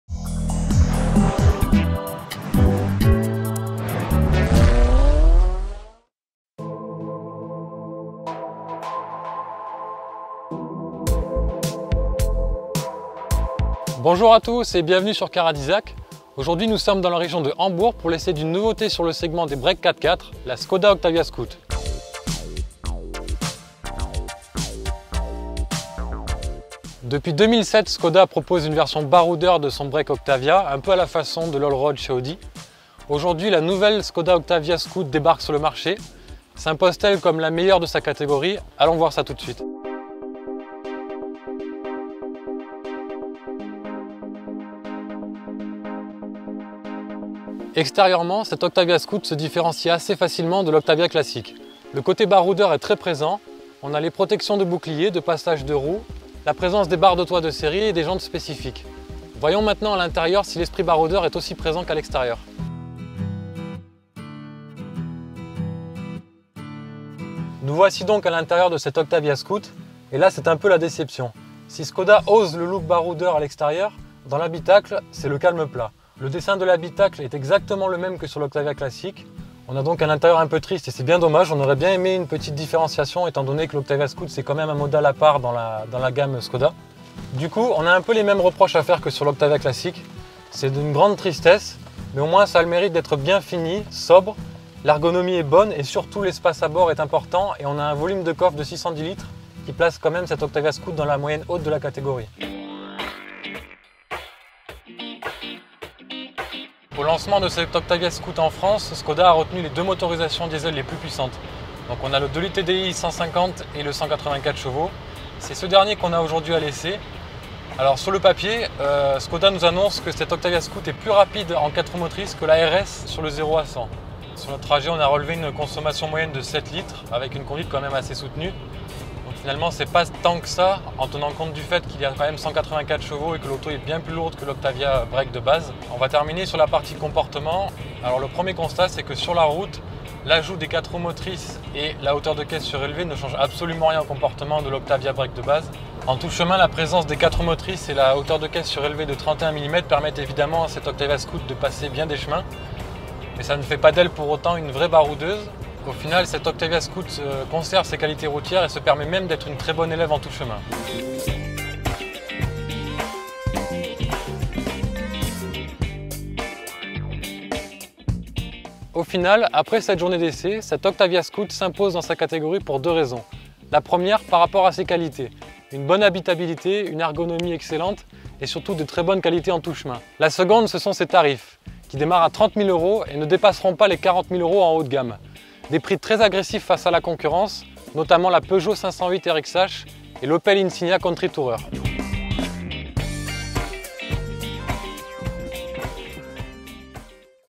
Essai Skoda Octavia Scout : offroad un jour, offroad toujours
« Scout un jour, scout toujours », dit l'adage. Chez Skoda, le terme est appliqué à l'Octavia qui offre une version offroad, l'Octavia Scout. Cette dernière est un peu l'élève qui fait bande à part dans la gamme Octavia, avec la RS. Skoda renouvelle aujourd'hui le break baroudeur avec cette Octavia Scout de seconde génération qui va faire mal à la concurrence. Explications.